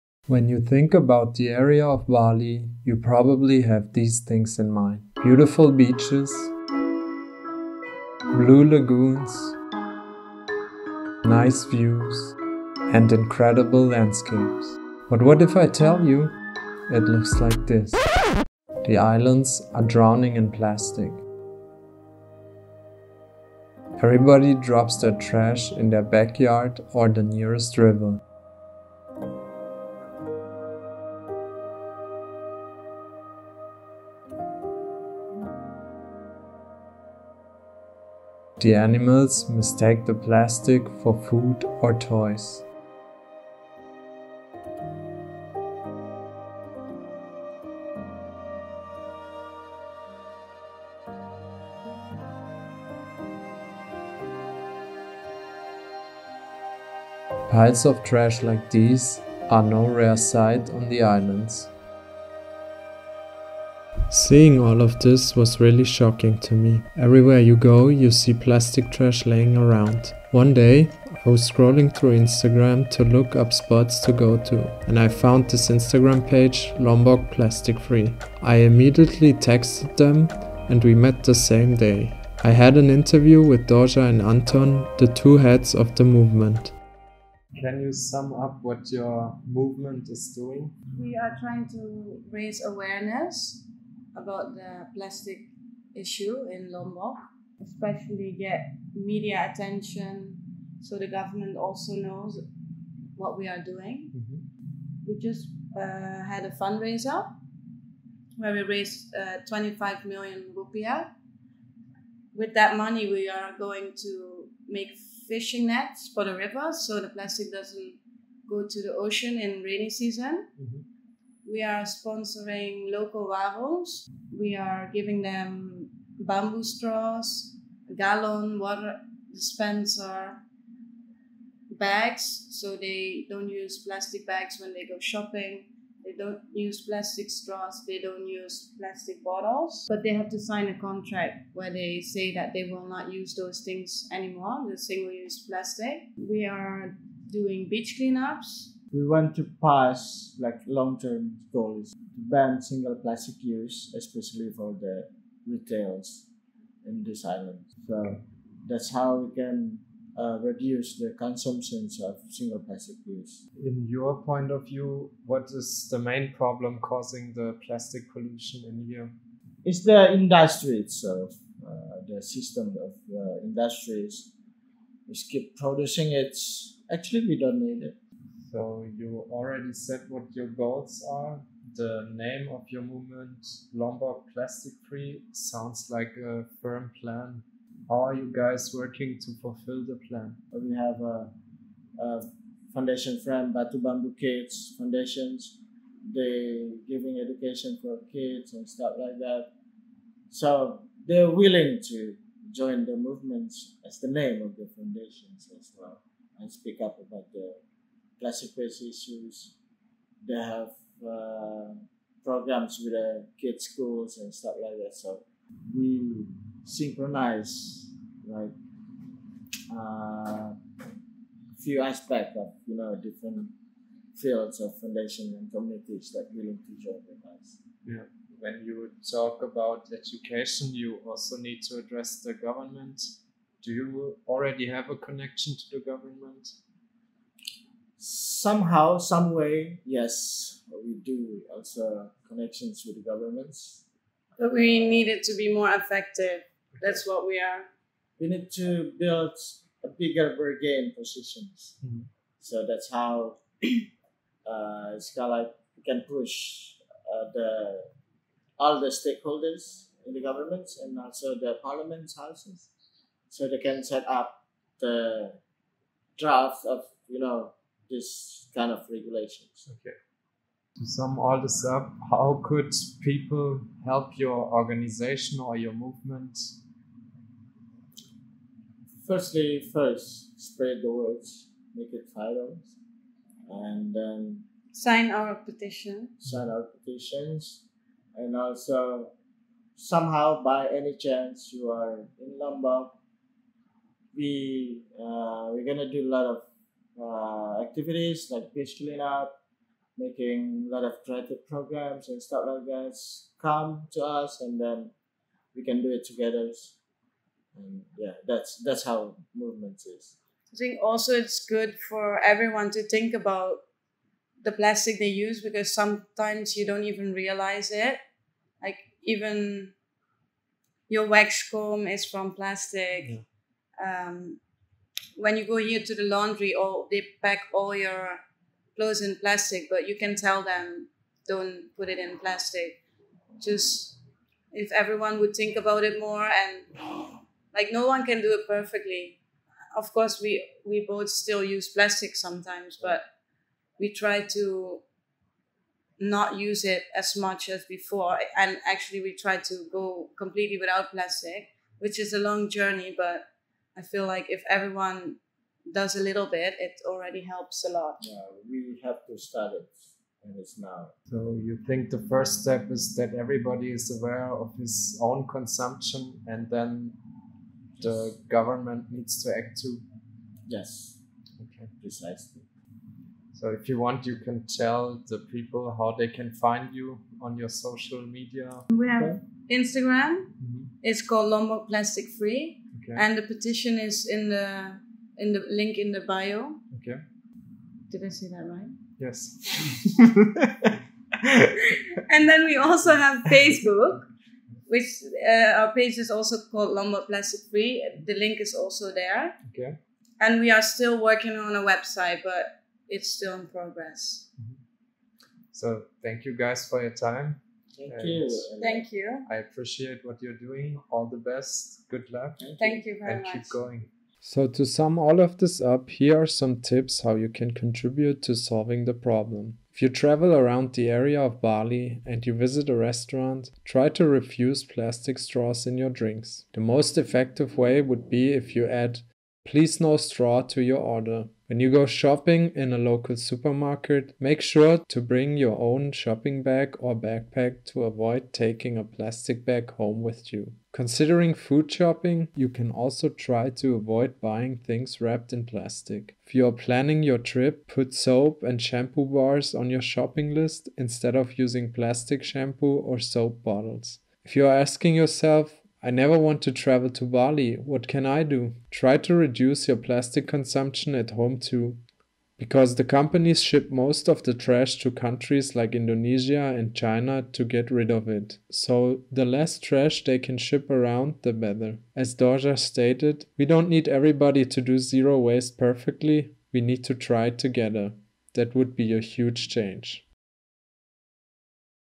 Plastic Trash - The truth about Lombok/Bali - Lombok Plastic Free (Documentary)
In this documentary, I will show you the reality in Lombok and Bali. With this Documentary, I want to raise awareness about the fact, that the islands around Bali are drowning in Plastic Trash in the Ocean. Therefore I interviewed the movement Lombok Plastic Free. At the end of the video, I show you some single use plastic alternatives.

How will you help? let's discuss in the comments

It would be nice to hear what you like and disliked about my video concept.

Check out Lombok Plastic Free and their friends Batubambu on Instagram:
Lombok Plastic Free: @lombokplastikfree
Batu Bambu: @batubambu_lombok